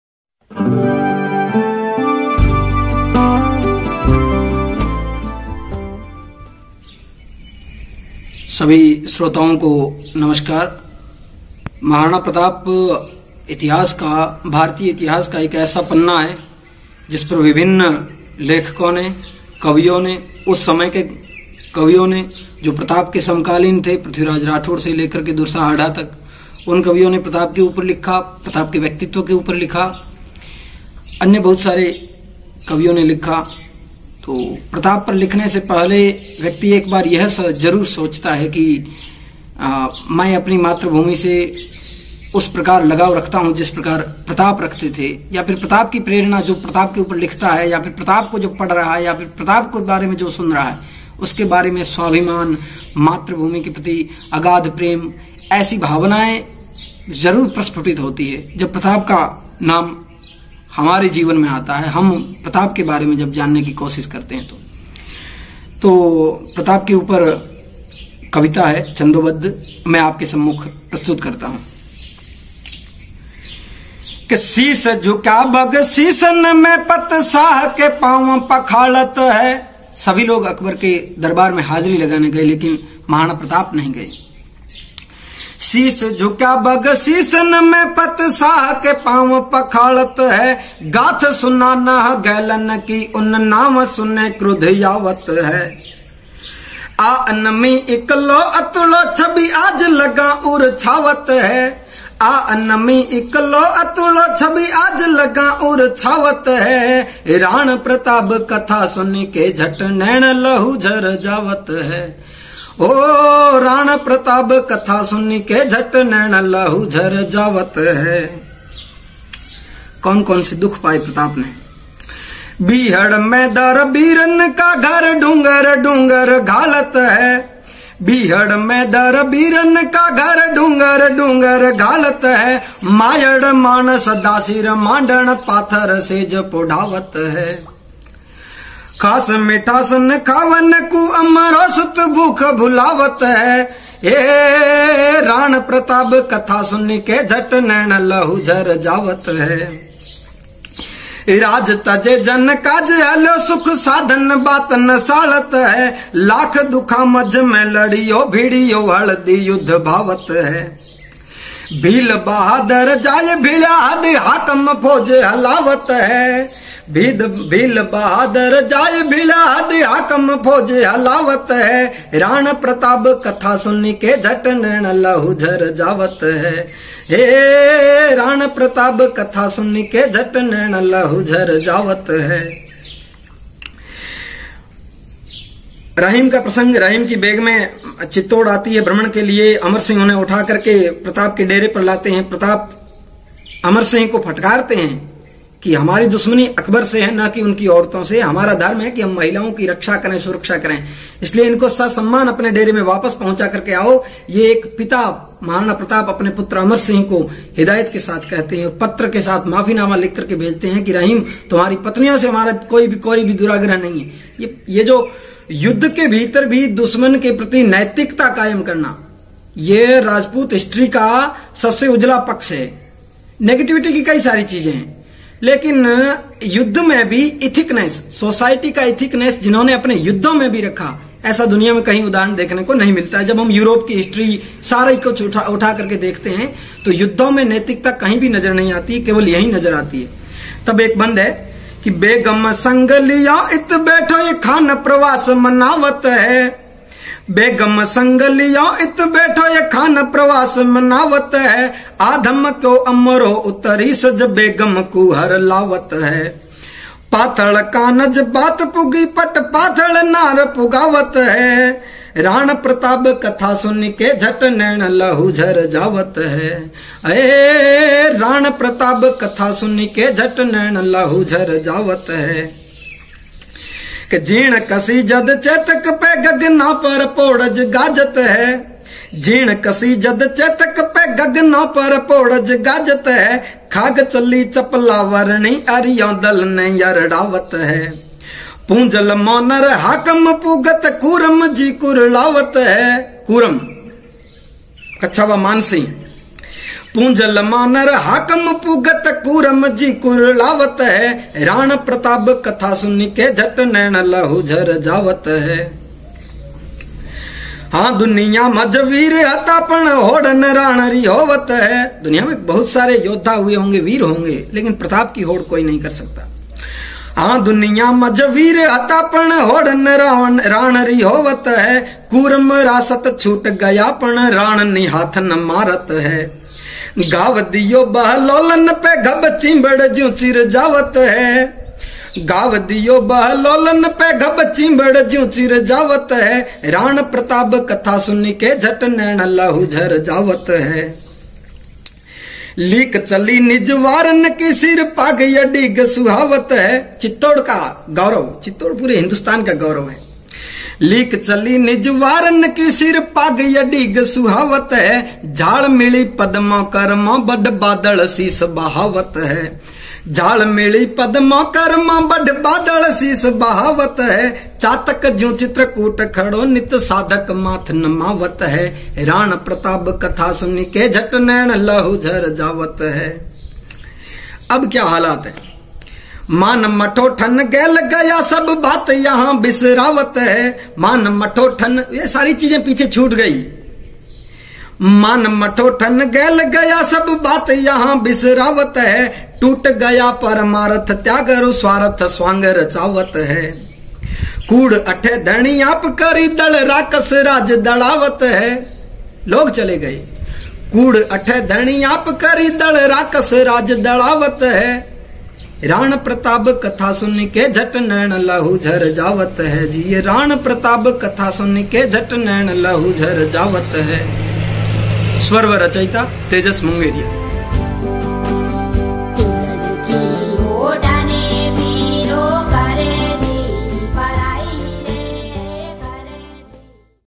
कविता | नैण लहू झर जावत है - महाराणा प्रताप यशोवर्णन | कवि तेजस मुंगेरिया | भाषा - ड़िंगळ राजस्थानी
मरु भूमि का उभरता कवि तेजस मुंगेरिया अपने छंदों के माध्यम से महाराणा प्रताप का यशोवर्णन इस प्रकार करता है .

जब जब हम महाराणा प्रताप की कथा सुनते हैं तो आँखों मे लहू भर जाता है और मातृभूमि के लिए अपना सर्वस्व त्यागने के लिए तीव्रता से प्रेरित हो उठते हैं । नश्वर देह की रूह थरथरा उठती है , देह के बाल शूलों की भांति खड़े हो जाते हैं । मानो देह, युद्ध मे मैदान में पहना जाने वाला शूलधारी कवच हो ।

'राण प्रताब कथा सुनिकै'
--मदिरा सवैया--- ________________
सीस झुक्या बगसीसन में
पतशाह के पांव पखाल़त है।
गाथ सुना नह गैलन की,
उन नांव सुनै क्रुद्ध आवत है।
आ अनमी इकलौ अतुळौ,
छबि आजलगां उर छावत है,
झट नैण लहू झर जावत है।।

बीहड़ में दर बीरन का,
घर डूंगर-डूंगर घाल़त है।
मायड़ माण सदा सिर मांडण,
पाथर सैज पोढ़ावत है।
खास मिठास न खावन कौ,
अमरौ सुत भूख भुलावत है।

राज तजै जन काज हल्यौ,
सुख साधन बात न साल़त है।
लाख दुखां मझ में लड़ियौ,
भिड़ियौ हल़दी युद्ध भावत है।
भील बहादर जाय भिळ्या,
हद हाकम फौज हलावत है।

बैगम संग लियां इत बैठोय,
खान प्रवास मनावत है।
आ धमक्यौ अमरौ उत रीस'ज,
बैगम कू हर लावत है।।
पाथल़ कान'ज बात पुगी,
पट पाथल़ नार पुगावत है।

जीण कसी जद चैतक पै,
गगनां पर पौड़'ज गाजत है।
खाग चली चपला वरणी,
अरियांदल़ नै अरड़ावत है।।
पूँजल ,मान'र हाकम पूगत,
कूरम जी कुरळावत है।

हां दुनिया मझ वीर हता पण,
हौड़ न राण की हौवत है।
कूरम रा सत छूट गया पण
राण निहाथ न मारत है।।
घाव दियौ बहलौलन पै घब
चींबड़ ज्यूं चिर जावत है।।

लीक चली निज वारन की,
सिर पाग अडीग सुहावत है।
झाळ मिल़ी पदमां, करमां
बढ़ बादळ सीस बहावत है।।
चातक ज्यूं चित्रकूट खड़ौ,
नित साधक माथ नमावत है।।

मान मठौठन गैल गया सह,
बात यहाँ बिसरावत है।
टूट गया परमारथ , त्याग'रु
स्वारथ स्वांग रचावत है।।
कूड़ अठै धणियाप करी,
दल़ राकस राज डरावत है।।

---तेजस मुंगेरिया